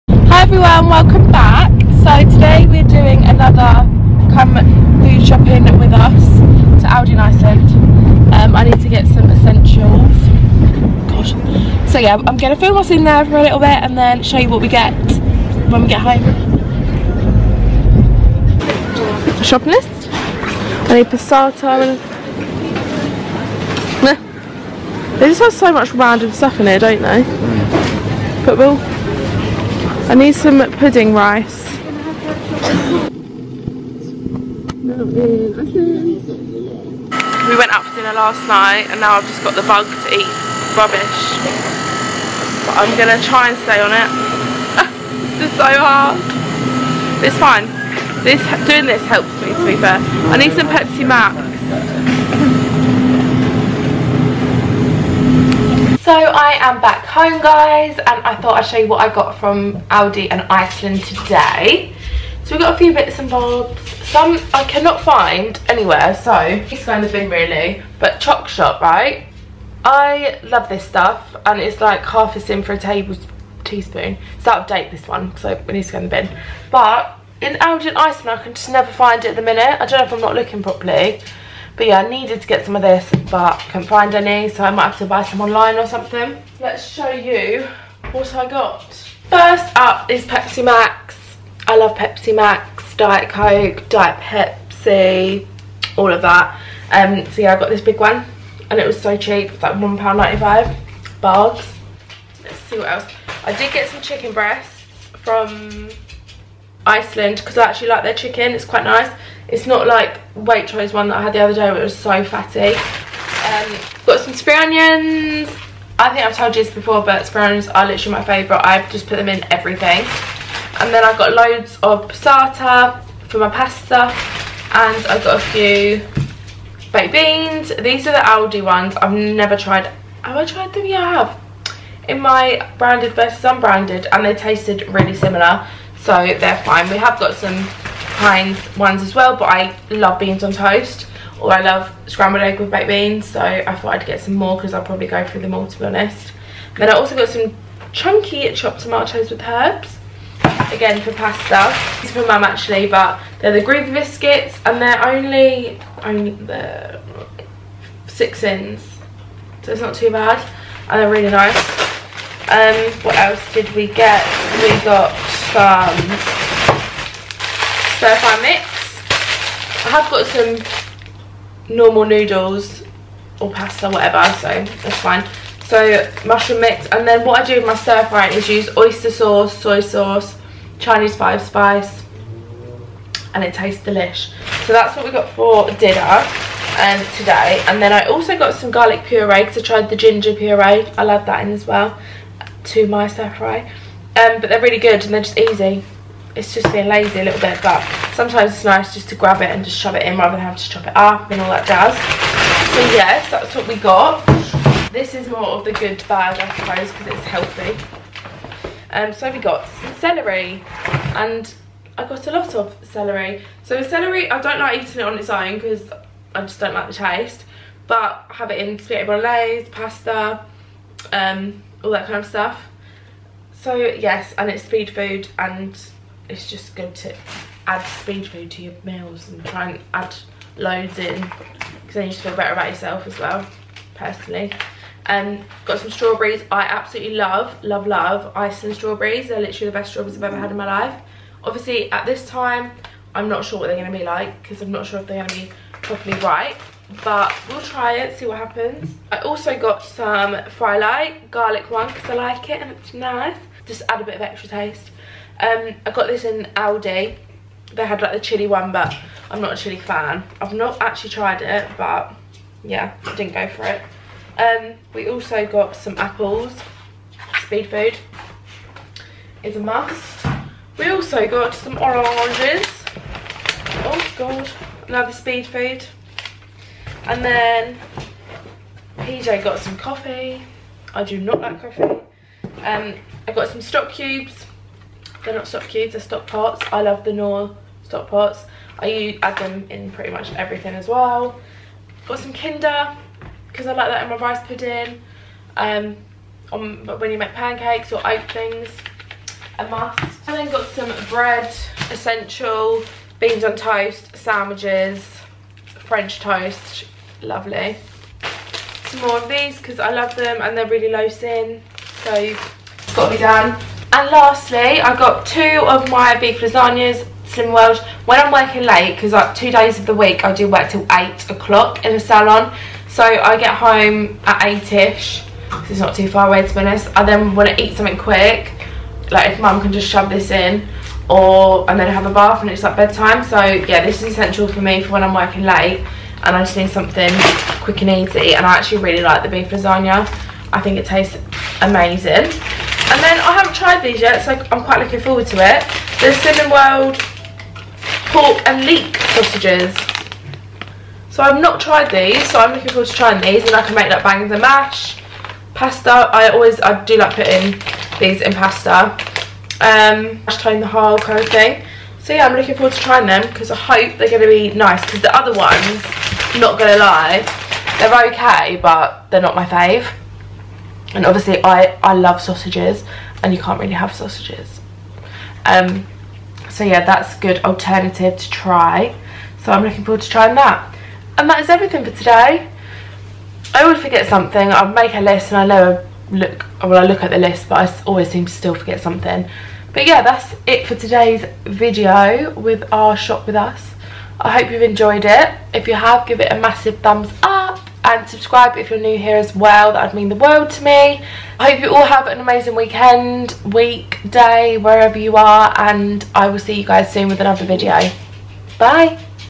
COME FOOD SHOPPING WITH US | SLIMMING WORLD FOOD HAUL | ALDI & ICELAND
Hello lovelies,
Here’s another come food shopping with us to Aldi and Iceland to get slimming world goodies/essentials!

Lydia xx